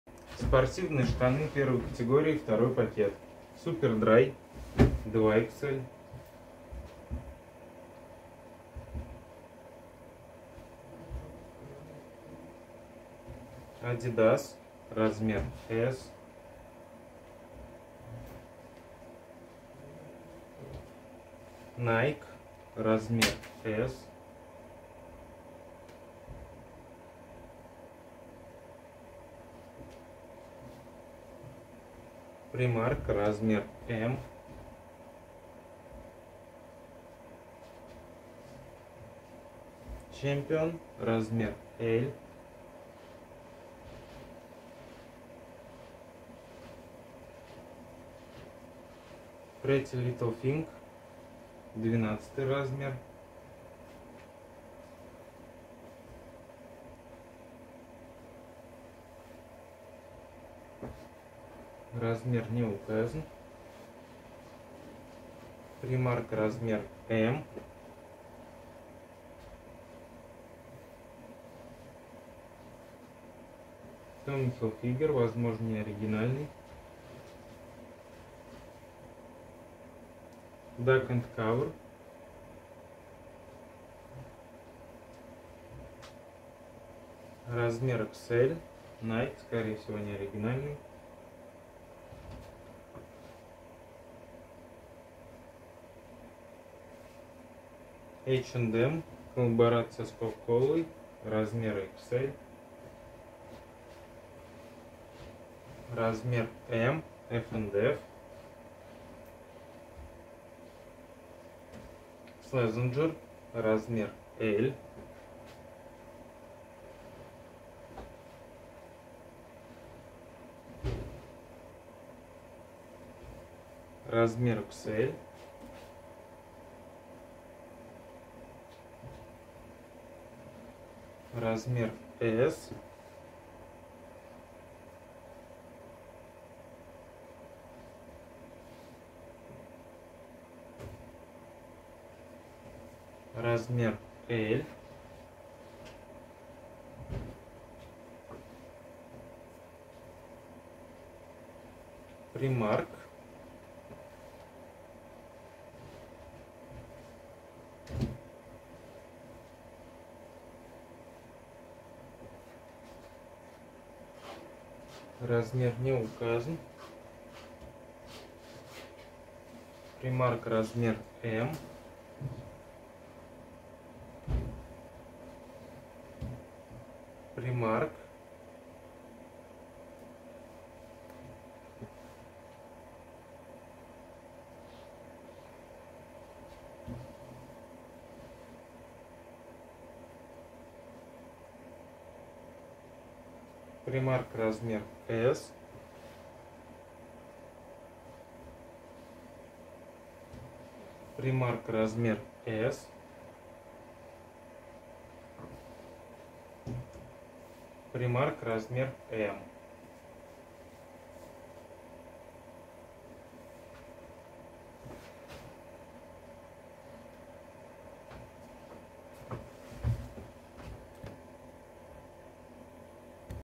34151 Штаны спортивные , 2пак, JOGGER, секонд (1 кат) оптом
34151 Штаны спортивные (JOGGER), секонд (1 кат), Англия, 2 пак, ~10 кг, 6.20€/кг, ~24 шт *

* актуальную цену в рублях, точный вес и количество вещей смотрите в нашем прайсе: